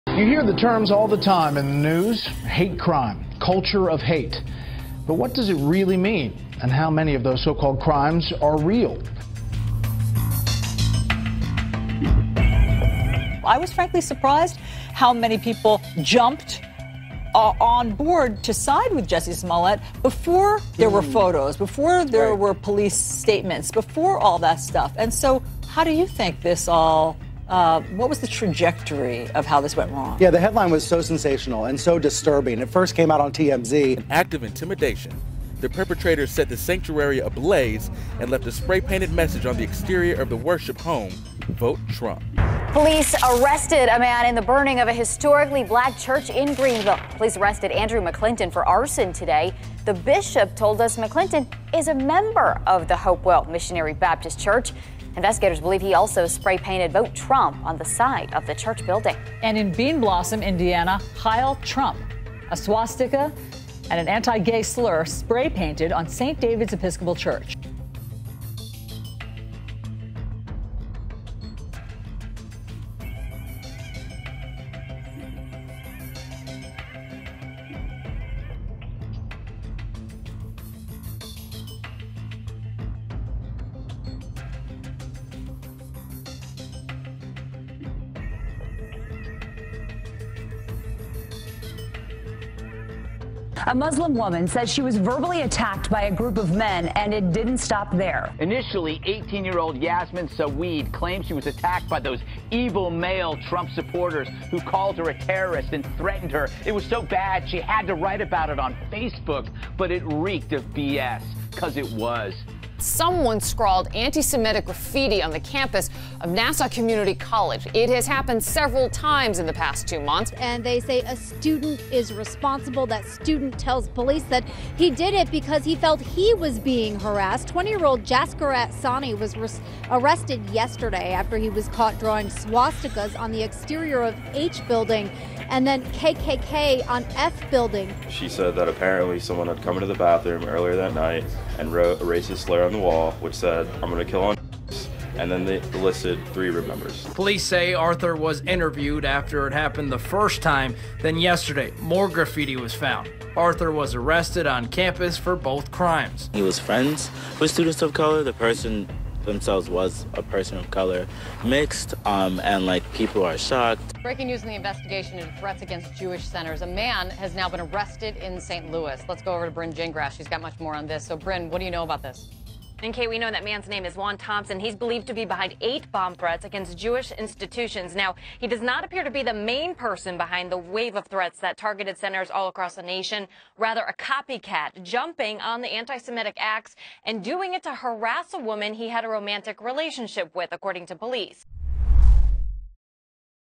Jussie Smollet Is Just Latest Instance Of Hate Crime Hoaxes
The media was quick to buy Smollett's bogus story about being assaulted by racist Trump supporters.  But this isn't the first time that happened.